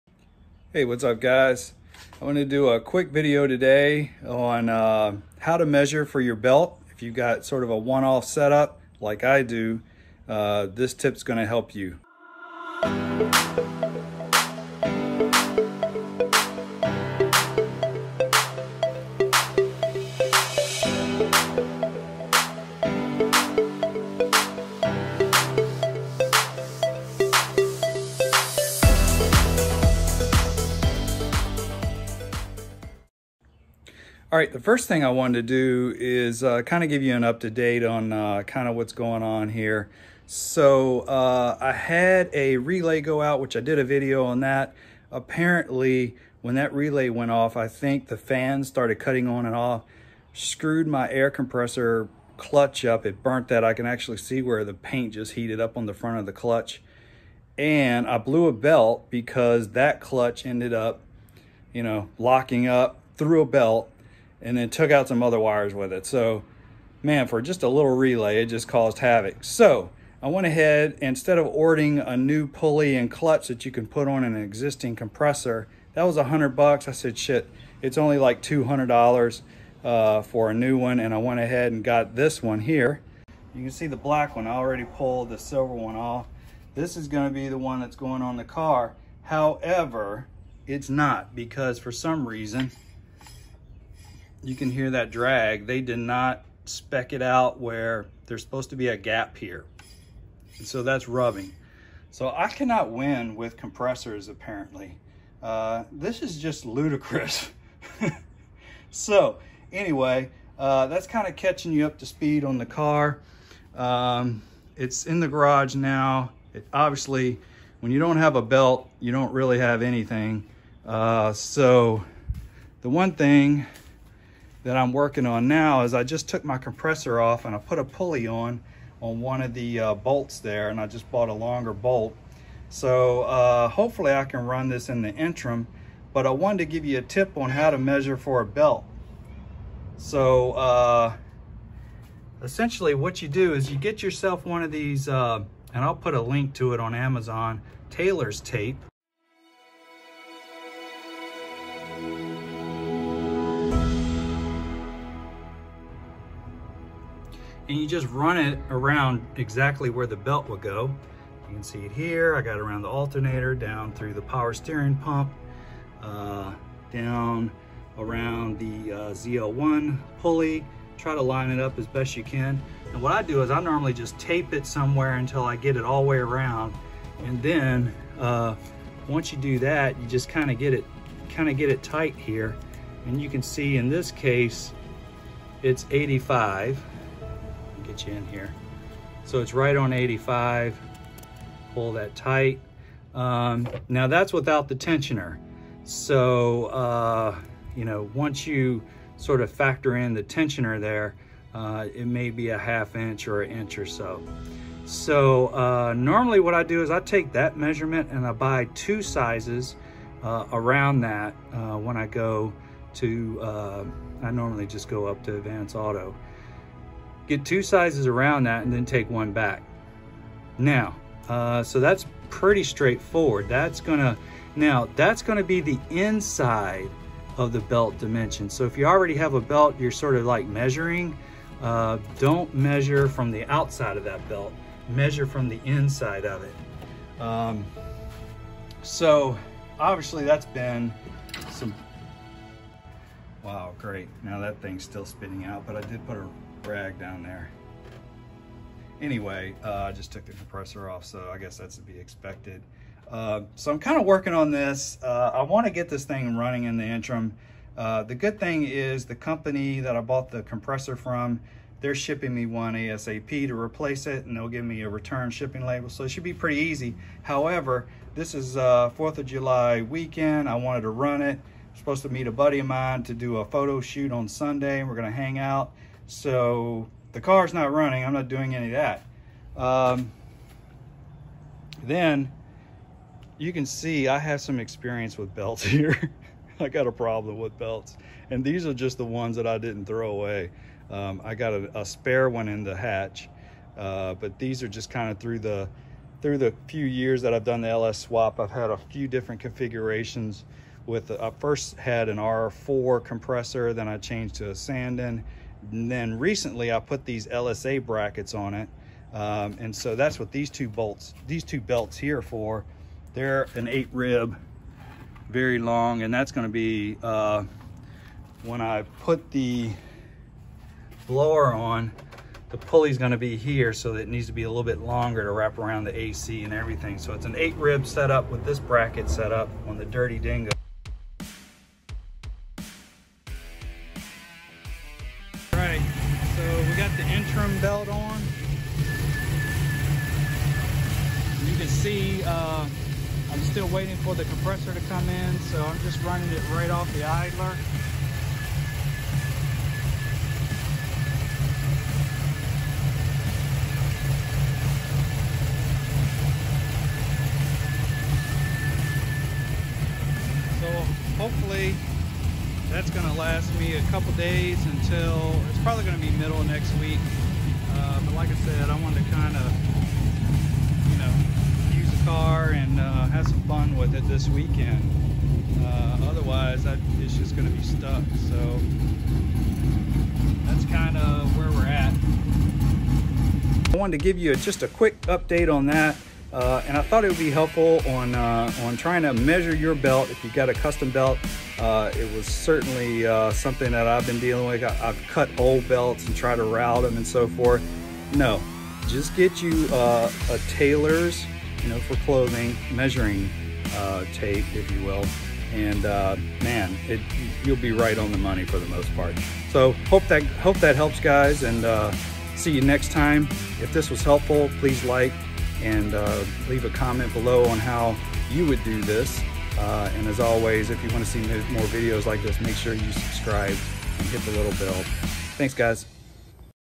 How to measure your belt! From LS Swap Basics series!
Belt measurements- Third GEN Camaro and Firebird LS Swap Basics series: I walk through how to measure for your custom length serpentine belt with a tailors measurement tape.